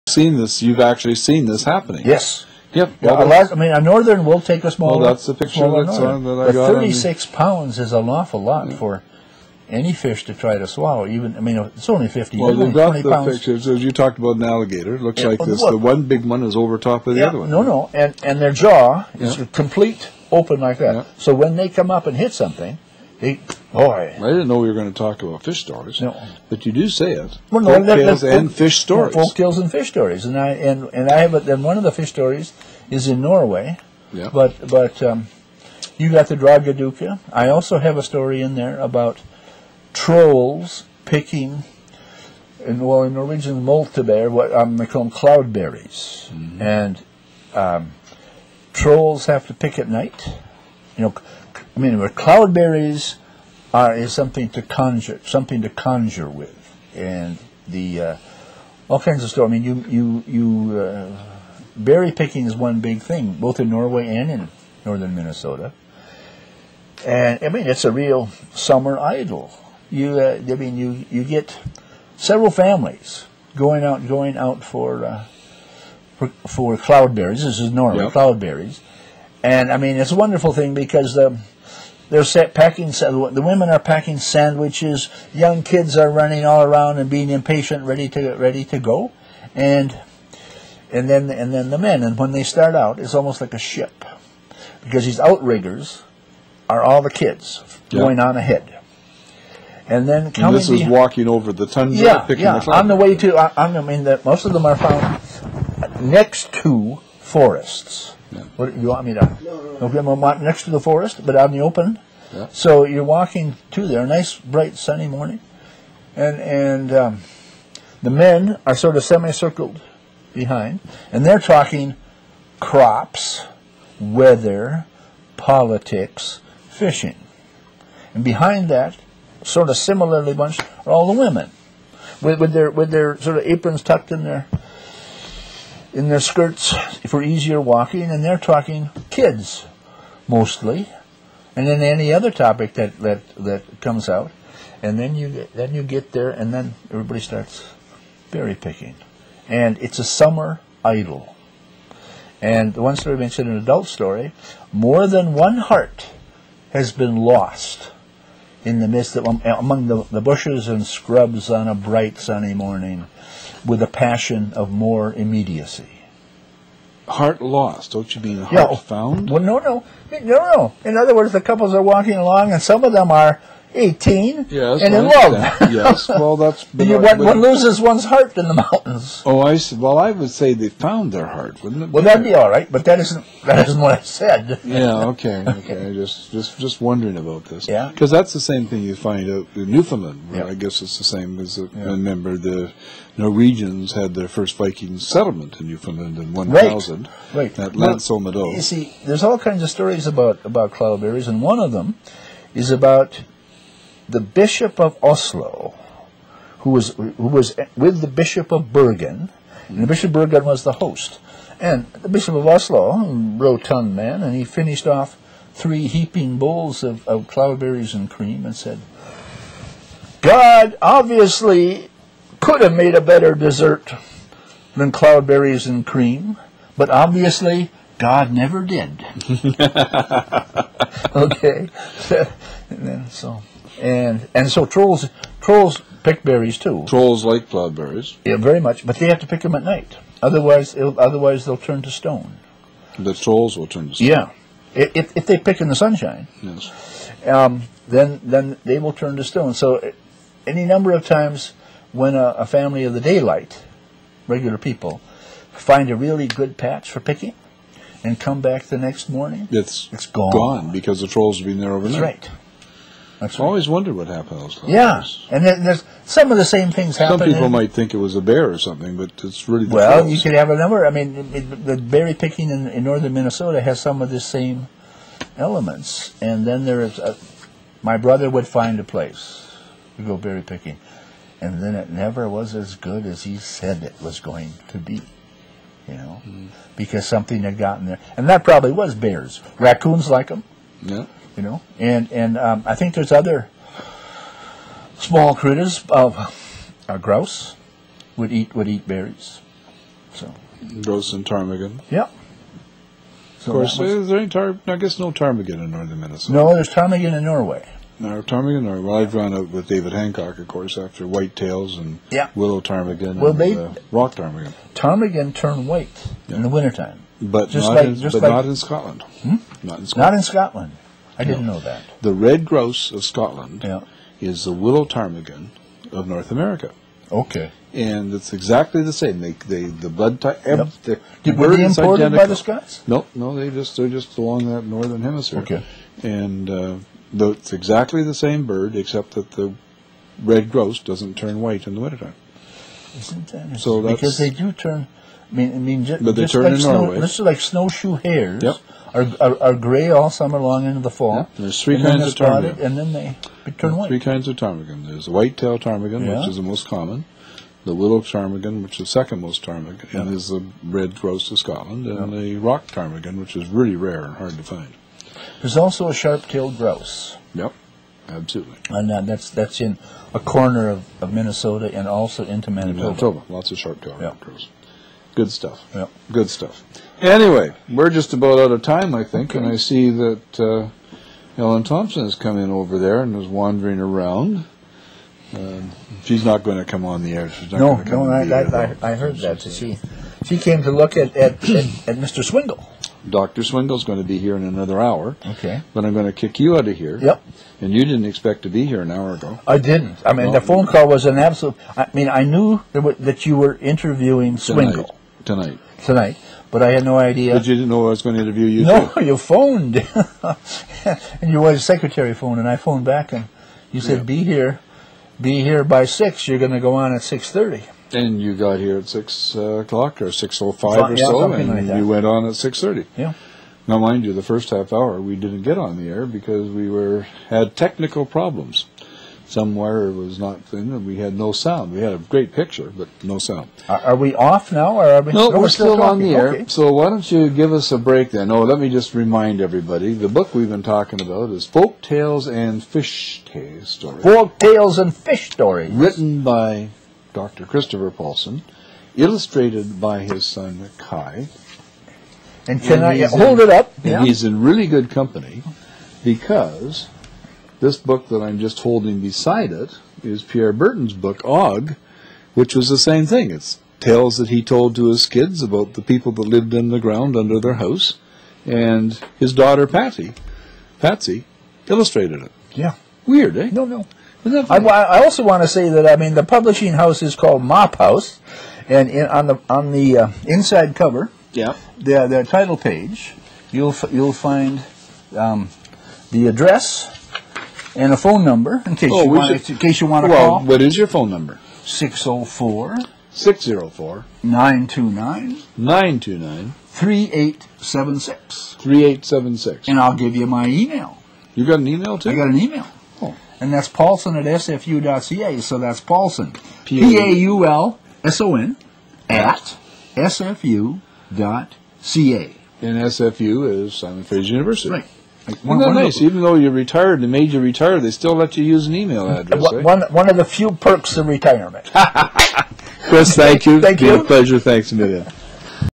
2009 March 18 3/15 - Kris Paulson
Kris Paulson and David talk about Kris' book Folk Tales and Fish Stories